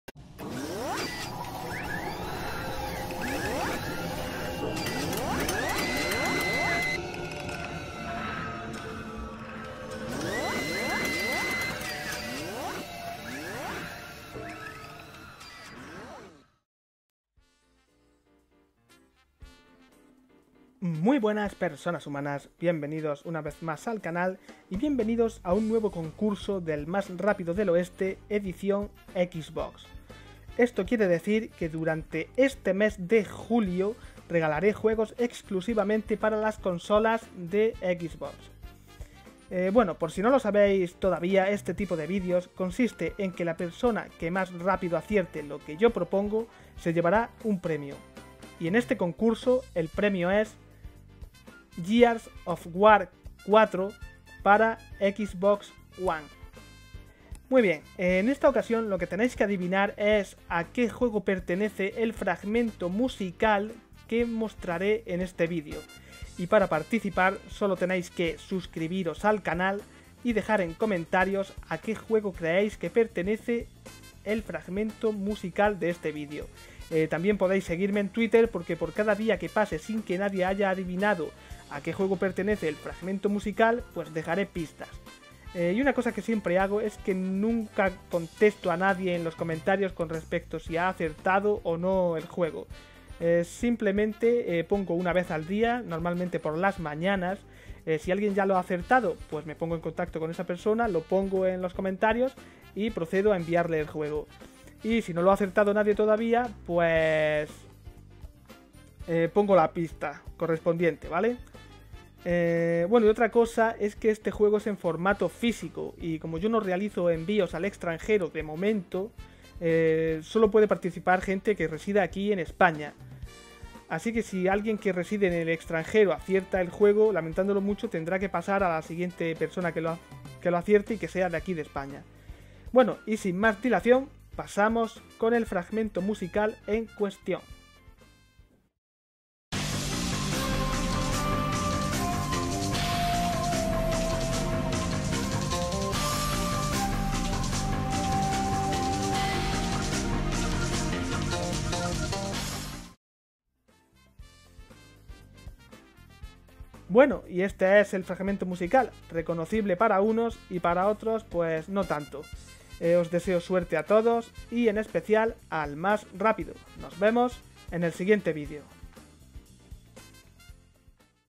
Concurso Gears of War 4 (Finalizado)
Pequeño concurso donde la persona que más rápido acierte a que juego pertenece el fragmento musical del vídeo se llevará una copia en formato físico de Gears of War 4 para Xbox One.

Los requisitos para participar son los siguientes:

2- Dejar en comentarios el nombre del juego al cual creáis que pertenece el fragmento musical.
3- Seguirme en Twitter para estar atentos a las pistas.

En este concurso solo pueden participar personas residentes en España.
Aclarar también que no se trata de ningún timo ni nada parecido, simplemente quiero hacer de Youtube un lugar mejor, donde todos puedan participar y ganar.